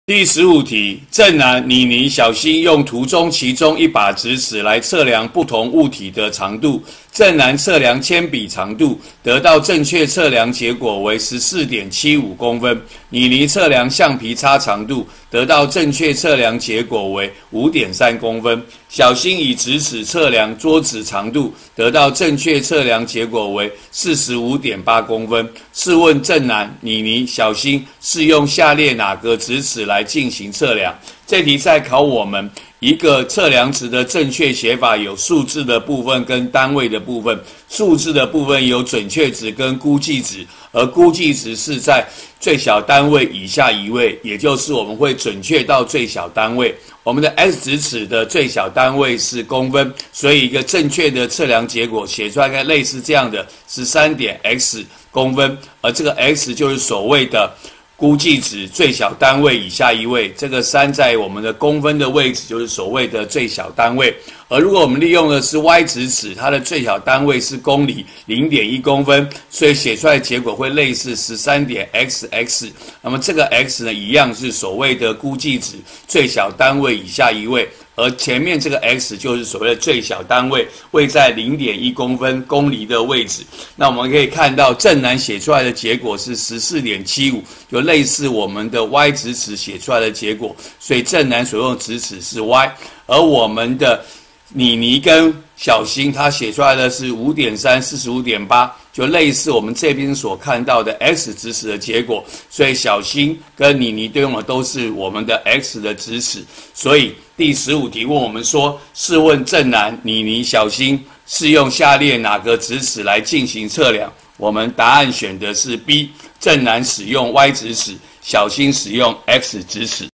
108學年上8年級理化一段第15題
兩把最小刻度不同直尺，給三人測量結果，問各人用哪一把直尺。